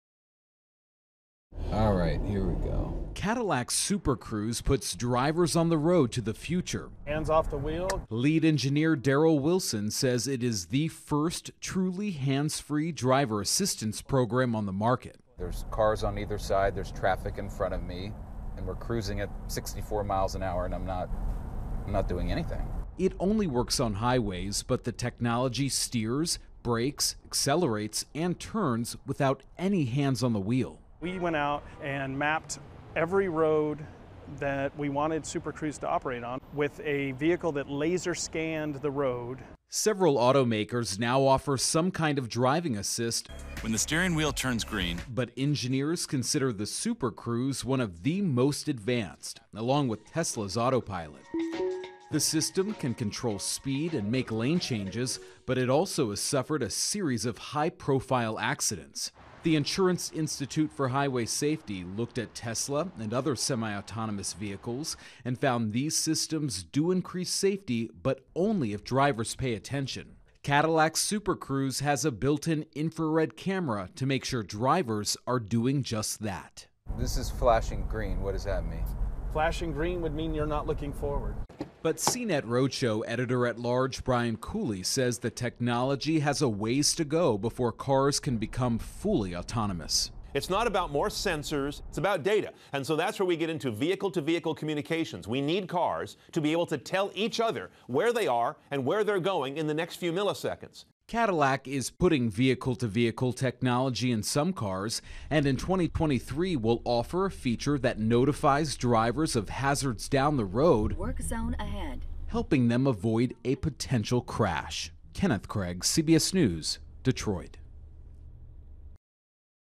Self Driving Cars Becoming A Reality In Some Showrooms
The race is on to build self-driving vehicles and automakers are one step closer. There are cars for sale in dealerships that you can drive hands free on the highway.